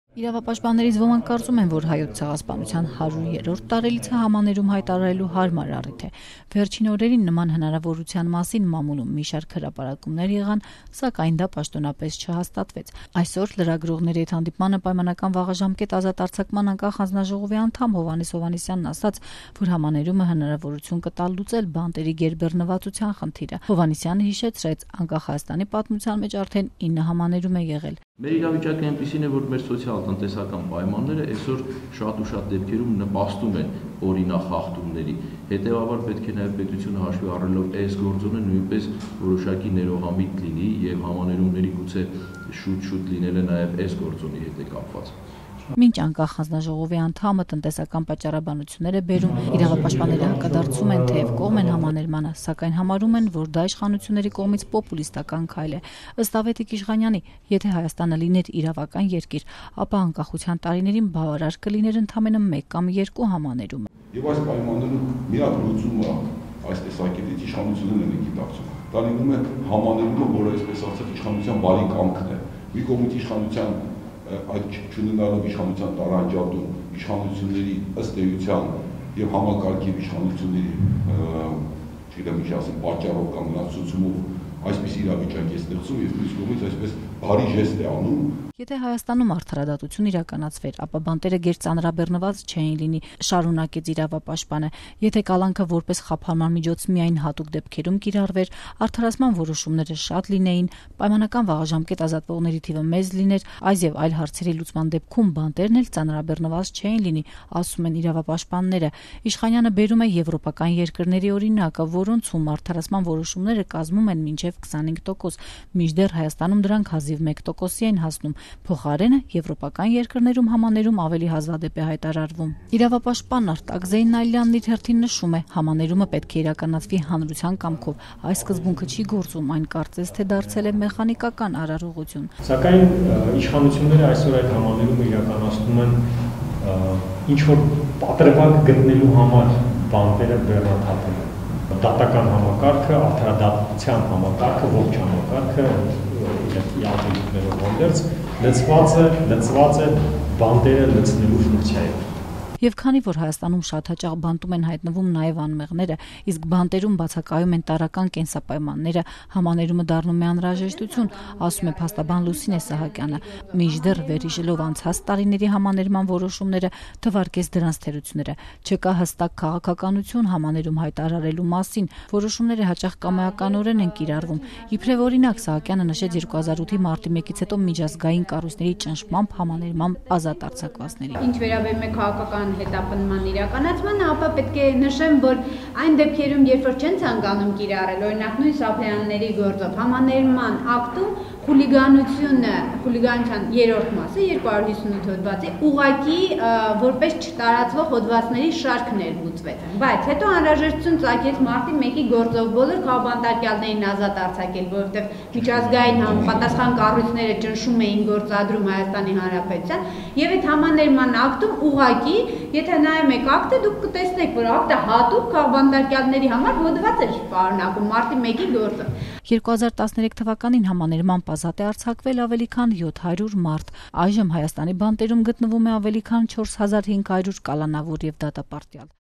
Համաներումը իշխանությունների կողմից «պոպուլիստական քայլ կլինի»
Իրավապաշտպաններից շատերը, թեև կողմ են համաներմանը, այդուհանդերձ, համարում են, որ դա իշխանությունների կողմից պոպուլիստական քայլ կլինի։